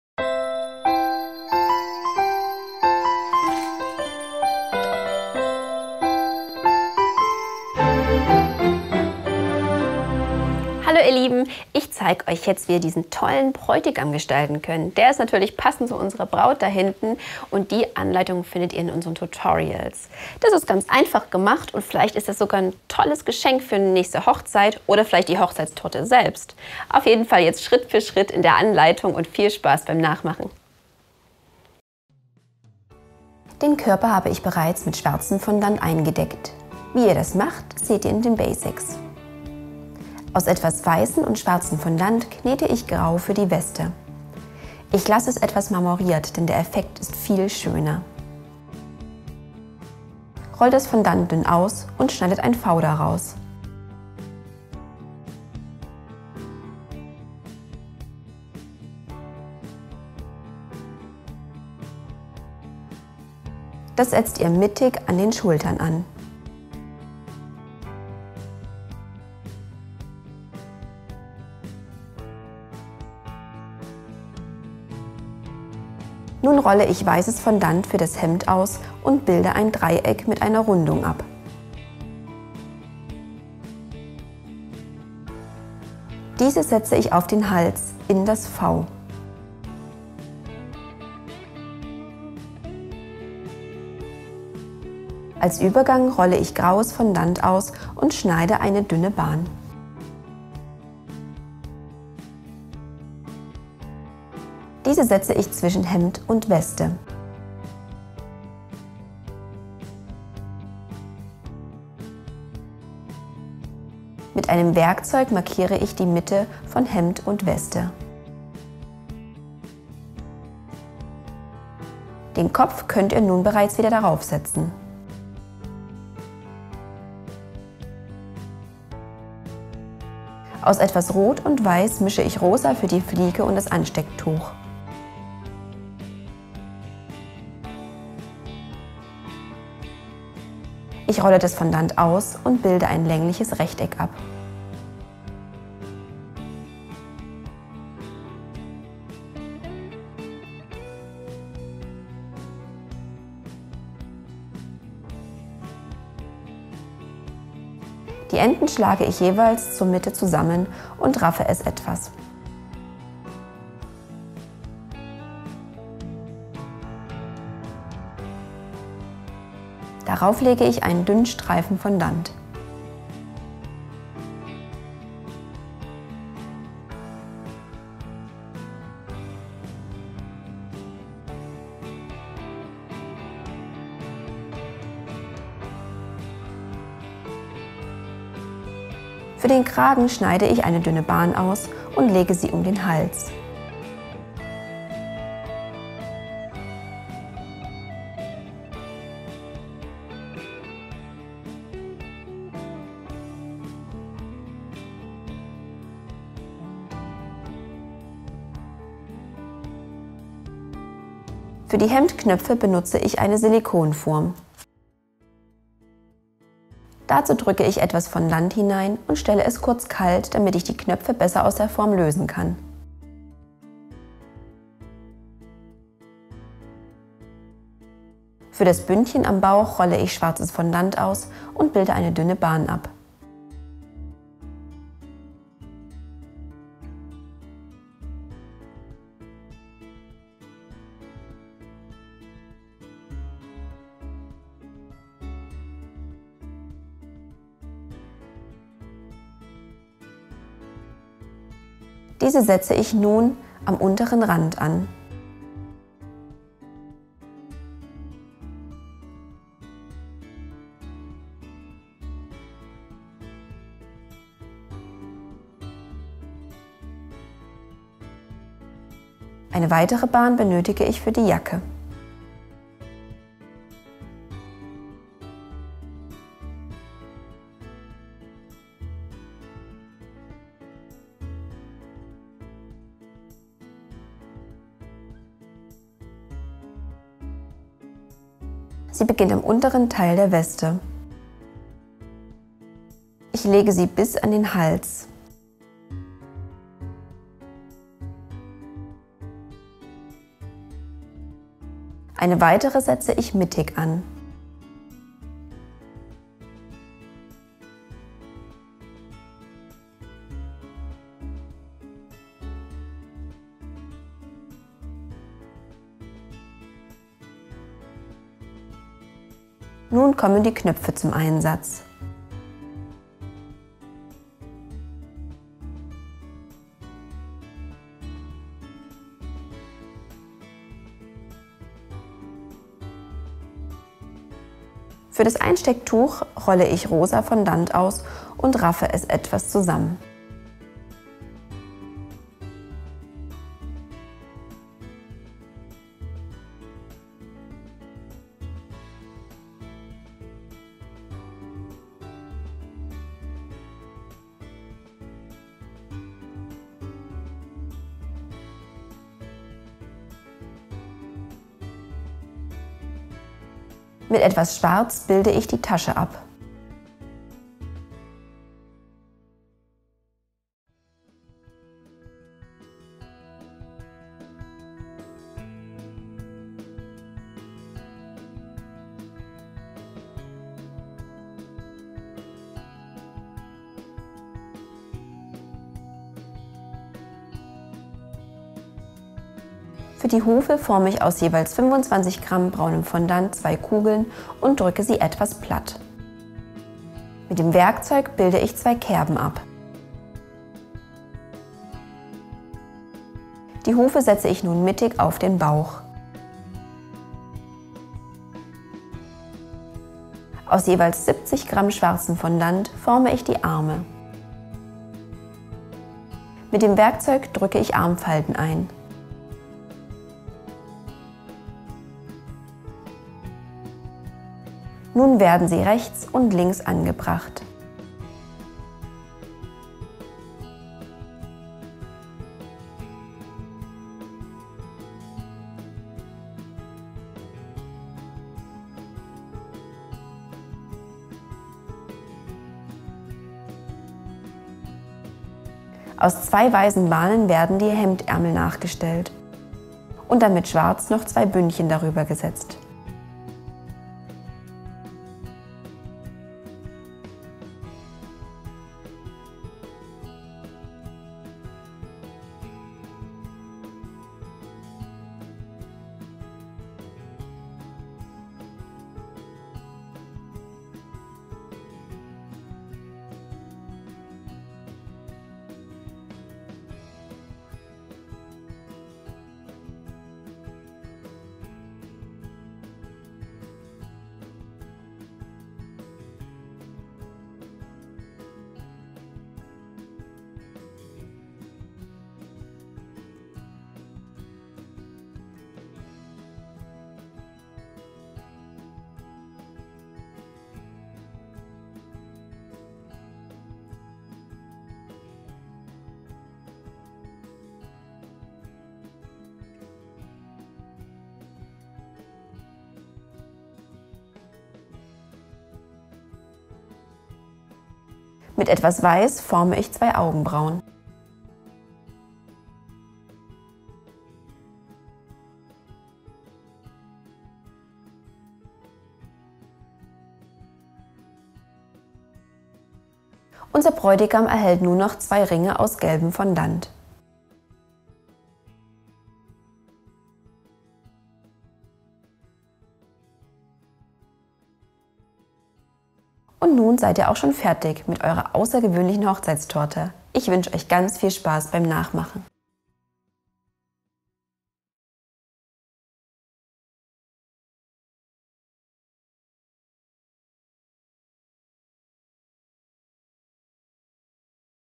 Caketales Torten-Anleitung: Verliebter Bräutigam
Wirklich ein süßes Paar! Das Gegenstück zu unserer hübschen Tortenbraut ist dieser liebenswerte Bräutigam. Als Geschenk zur Hochzeitsfeier, zum Junggesellenabschied oder zum Polterabend stiehlt er allen anderen die Show!
Wie Du diese tolle Motivtorte mit unserem Caketales Tortenständer (bekannt aus "Die Höhle der Löwen") selbst umsetzen kannst, zeigen wir dir in diesem Tutorial in Echtzeit - einfach mitmachen! Alle Tortenständer-Figuren, passende Kuchen- und Torten-Rezepte, sowie Ideen für herzhafte Snacks und süße Nachspeisen findest Du

Deko-Anleitung  •  mittel •  ca. 45 Min. Zubereitungs- & Dekozeit  •  auf alle Caketales Figuren anwendbar

Du brauchst dafür:

Zutaten & Dekomaterial:
- 400g schwarzes, 100g weißes Fondant
- ein wenig Rosa und 50g Braun für die Hufe
- etwas Goldpuder für die Ringe

Die Caketales Tortenständer sind im Set mit Backform und Etageren-Einsatz in unserem Online-Shop erhältlich!